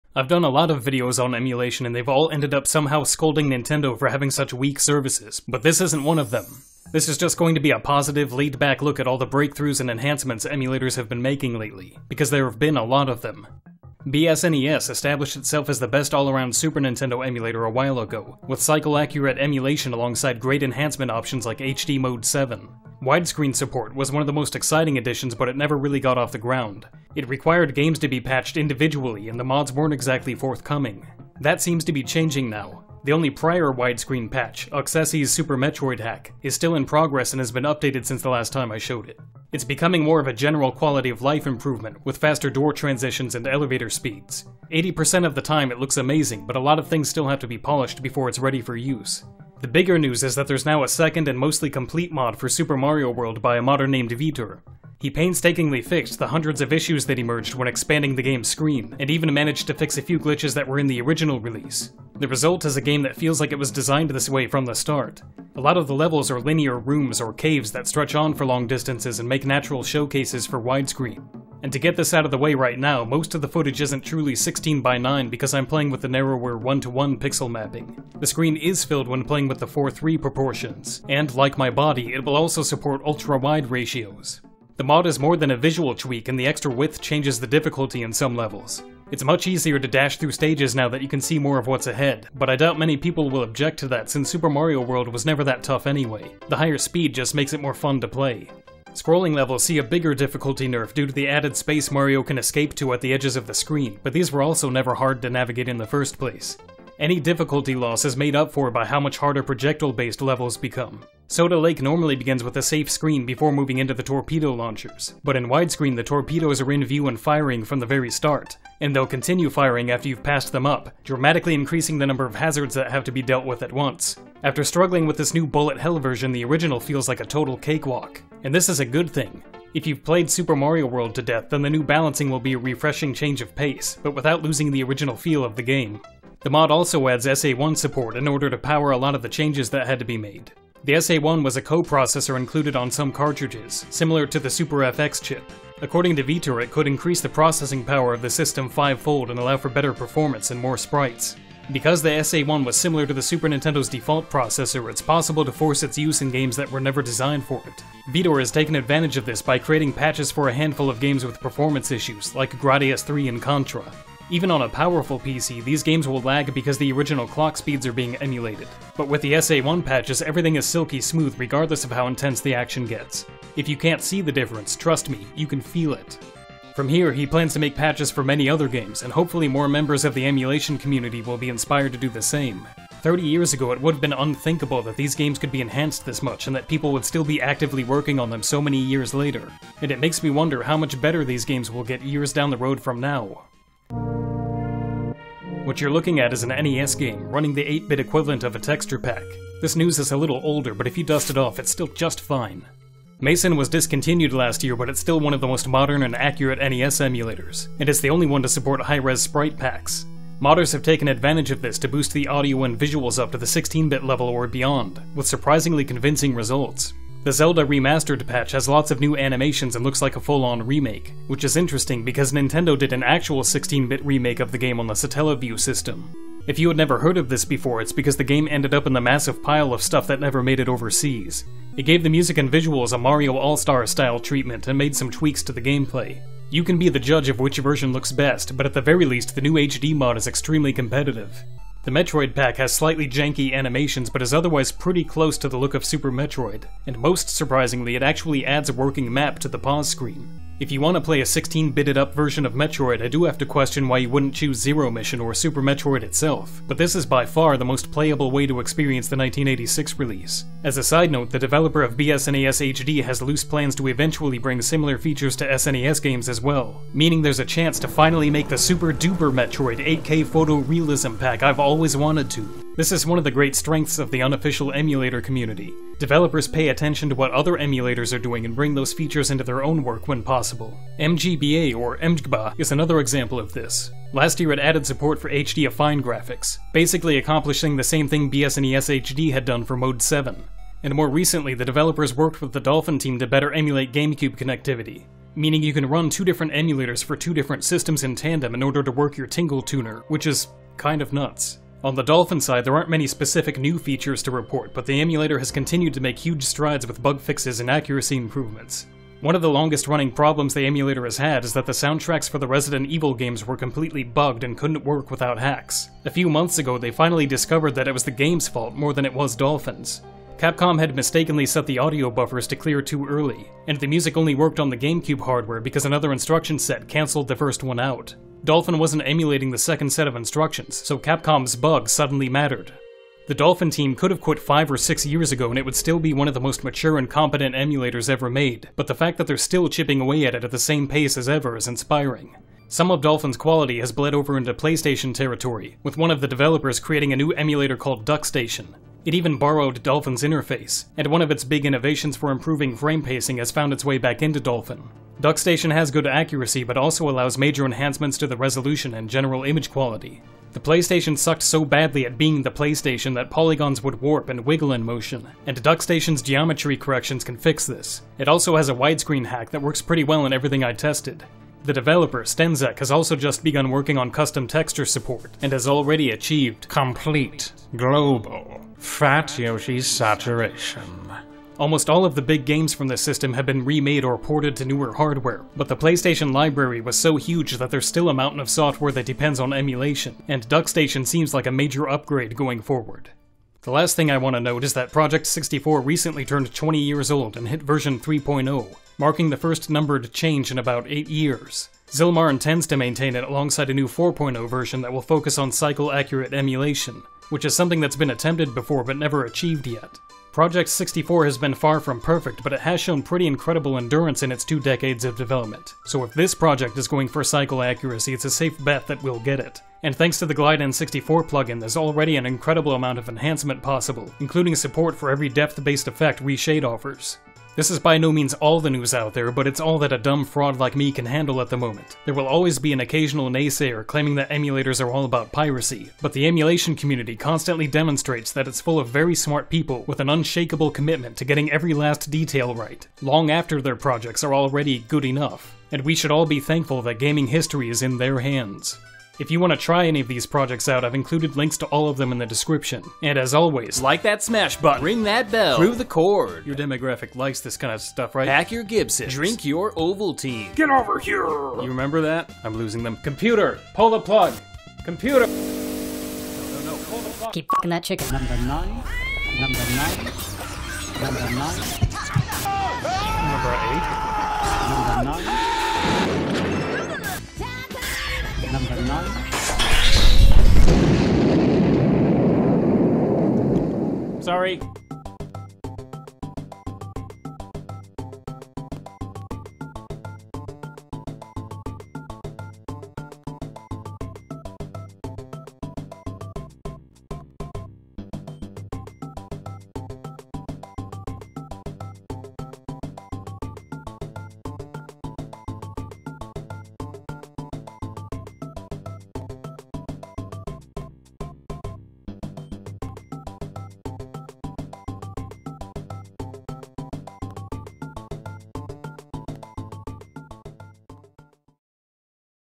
Emulators and Modders Continue to Kick Ass and I Want More, Daddy (No Safeword)
A brief look at the progress the fan emulation community has been making over the past year.

No games will be provided at these links, you'll need your own dumps: